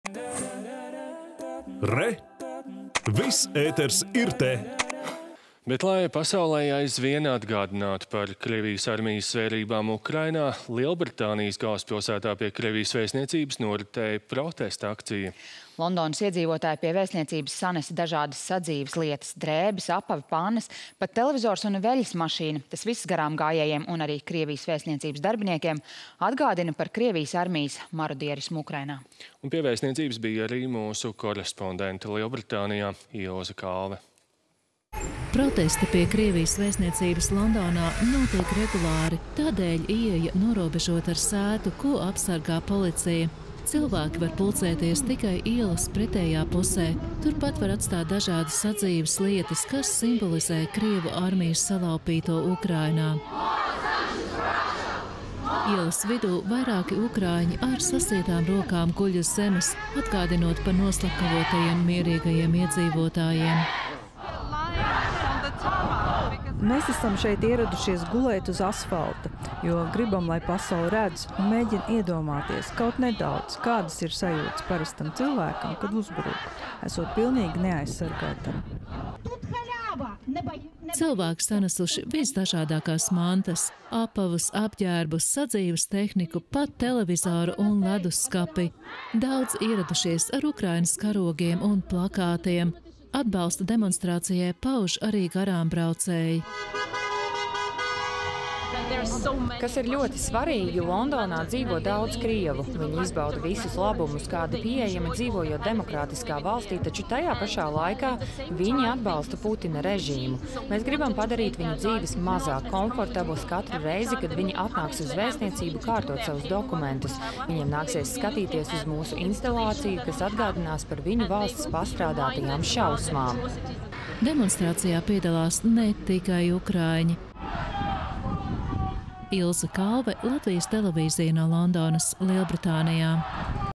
Londonā pie Krievijas vēstniecības sanes armijas laupīto
Lai pasaulei atgādinātu par Krievijas armijas zvērībām Ukrainā, Lielbritānijas galvaspilsētā pie Krievijas vēstniecības noritēja protesta akcija. Londonas iedzīvotāji pie vēstniecības sanesa dažādas sadzīves lietas. Drēbes, apavi, pannas, pat televizors un veļasmašīna – tas viss garāmgājējiem un arī Krievijas vēstniecības darbiniekiem atgādina par Krievijas armijas marodierismu Ukrainā. Pie vēstniecības bija arī mūsu korespondente Lielbritānijā Ilze Kalve.